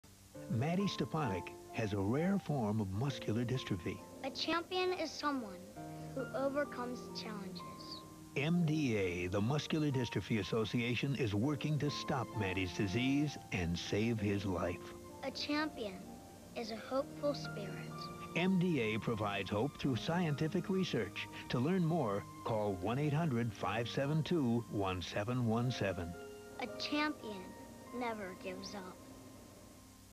MDA PSA w/Mattie Stepanek, 2002
Casey Kasem voices this spot in which the charitable organization's "National Goodwill Ambassador" makes a pitch for them.